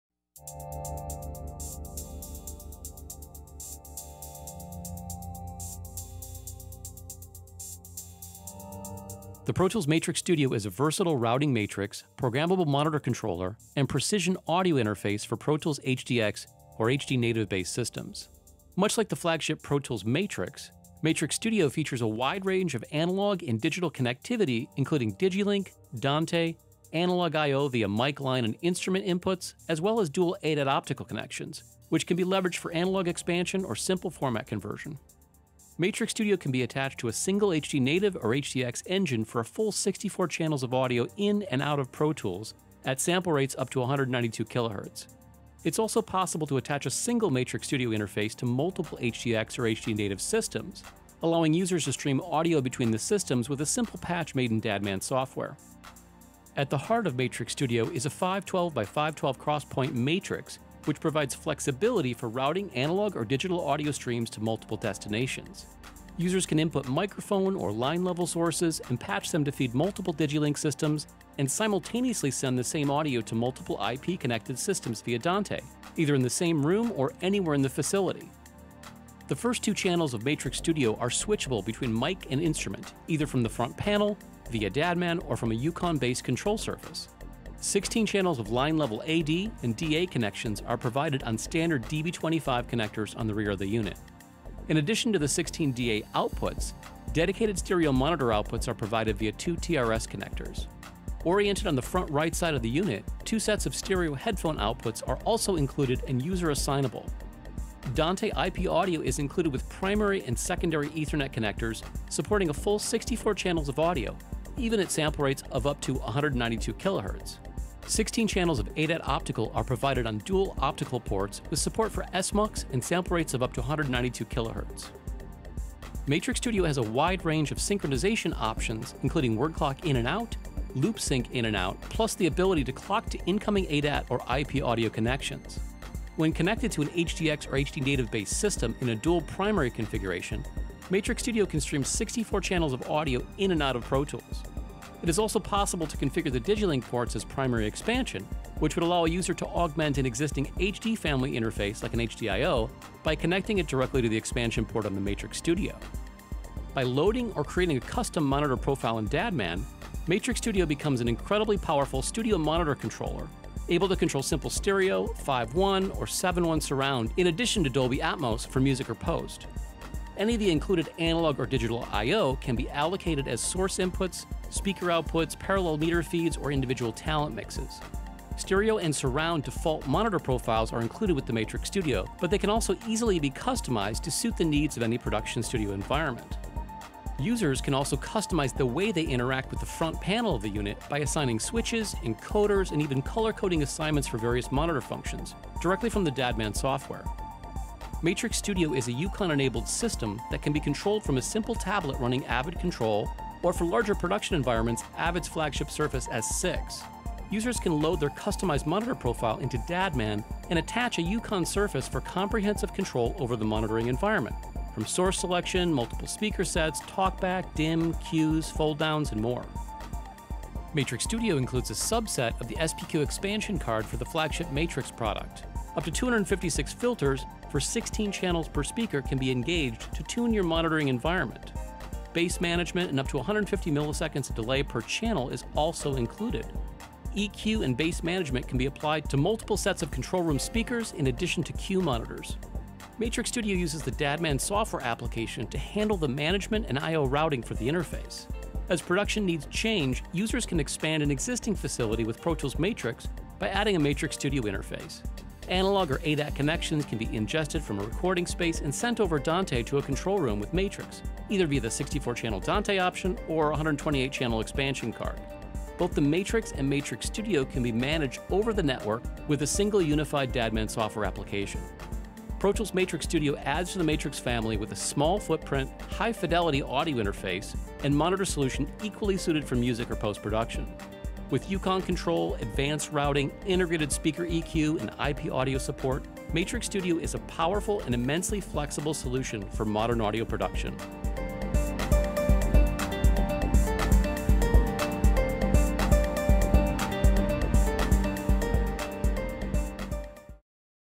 Pro Tools Tech Tip — MTRX Studio in Focus
Let's explore the operation and functionality of the Pro Tools | MTRX Studio